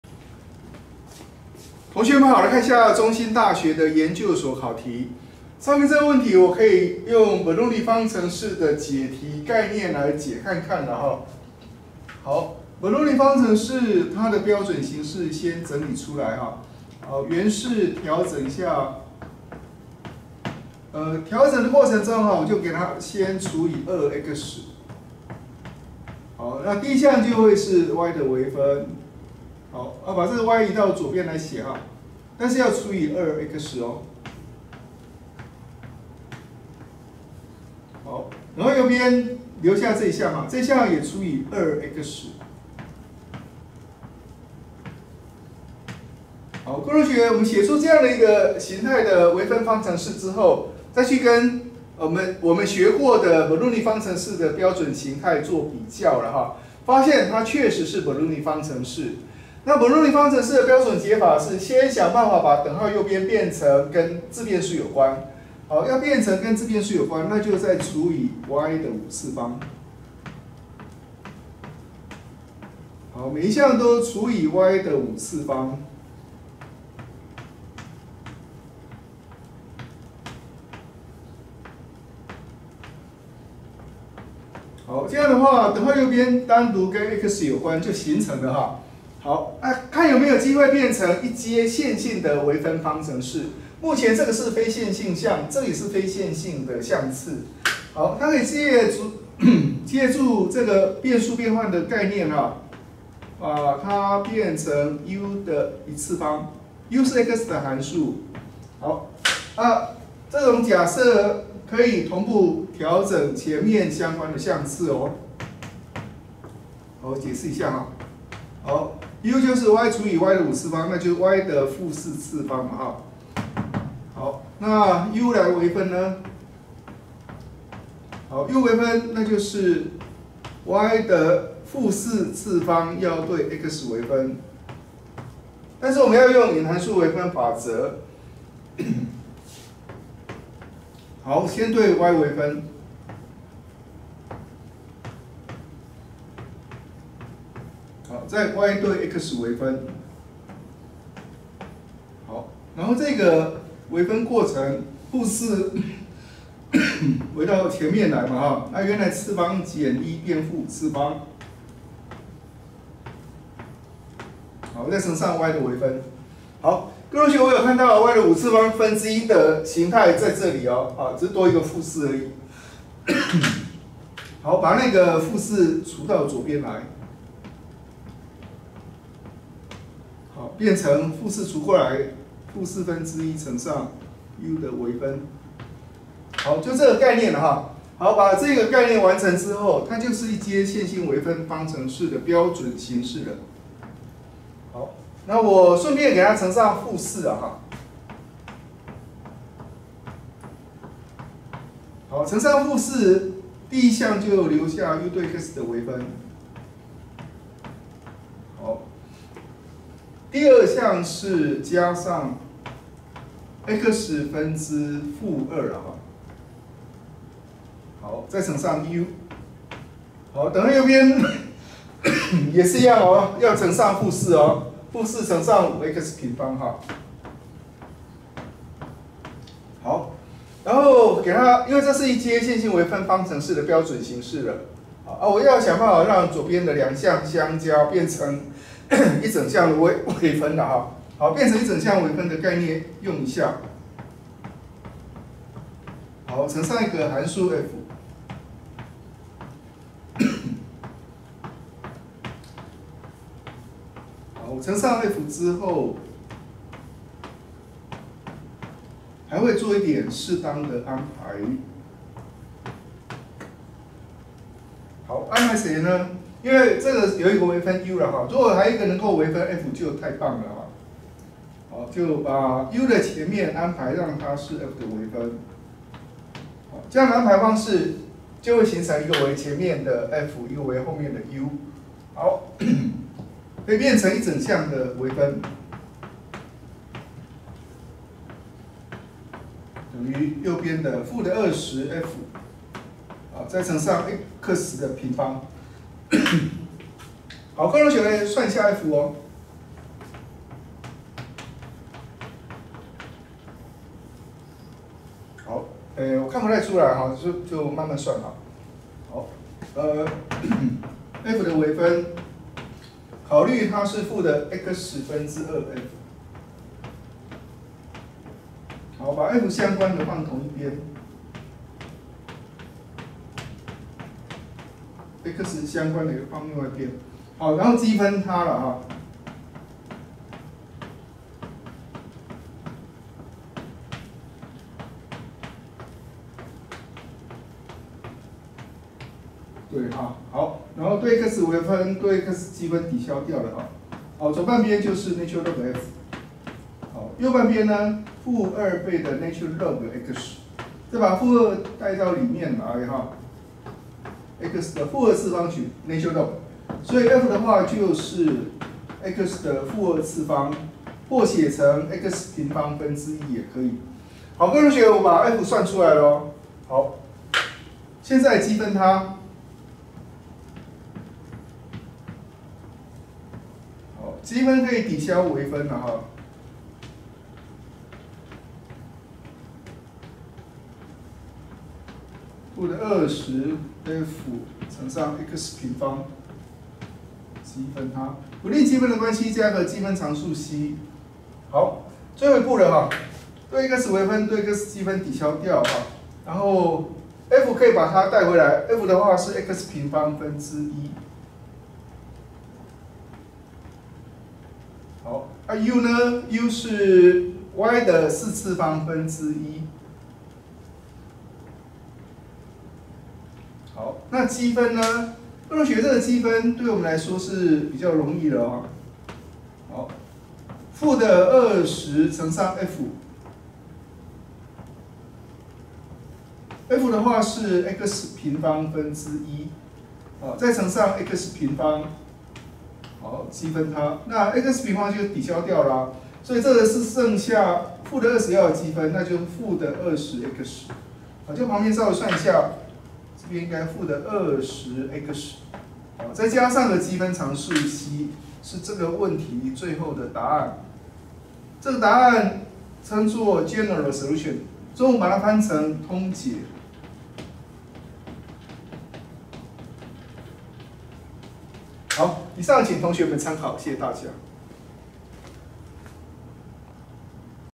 🏆【2018教育部教學實踐研究計畫】(4K) 2017中興：Solve 2xy’ = 10x³y⁵ + y 🔴提要015
【修訂】0:34起出現移項未變號之誤。正確解答為 x²/y⁴ ＝ ‒ 4x⁵ + C，其中 C 為任意常數， 請參閱解題講義。
本題擬採用 Bernoulli 方程式的解題精神，想辦法利用變數變換的觀念，將 Bernoulli 方程式簡化為一階線性微分方程式，再採用一階線性微分方程式的解題方法推求其解。